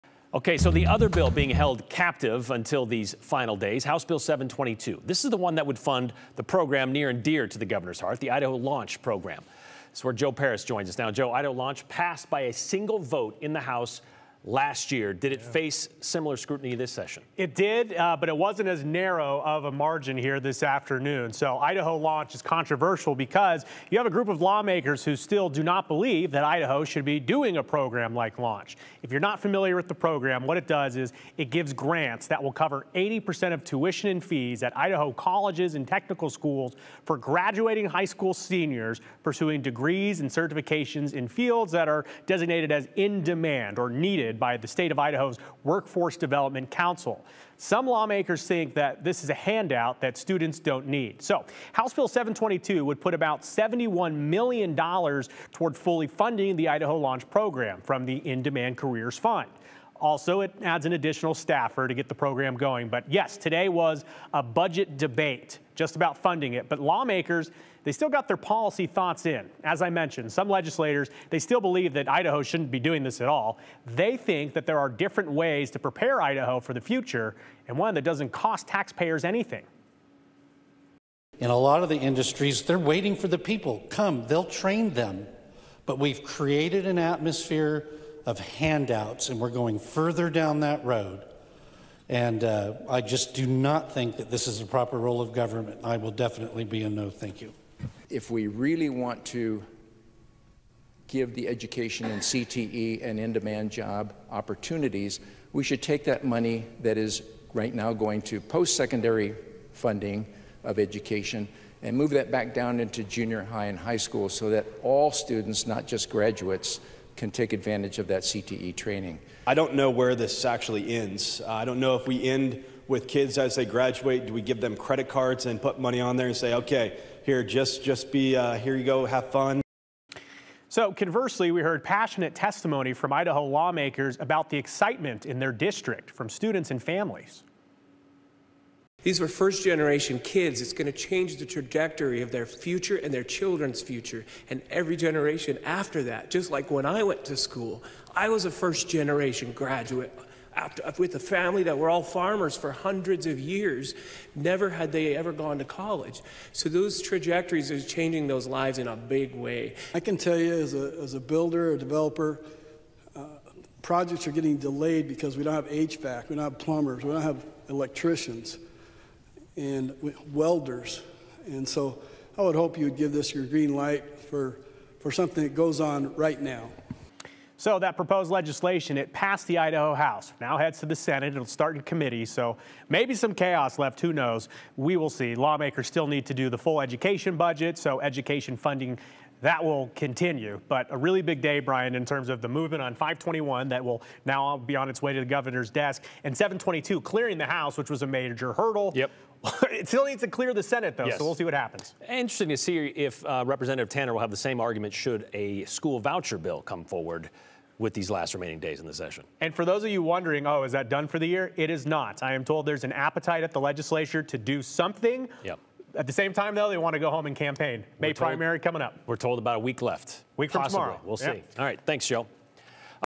House Bill 722: Idaho Launch Program clears the house
The grants will cover 80% of tuition and fees at Idaho colleges and technical schools for careers considered "in-demand" by Idaho's Workforce Development Council.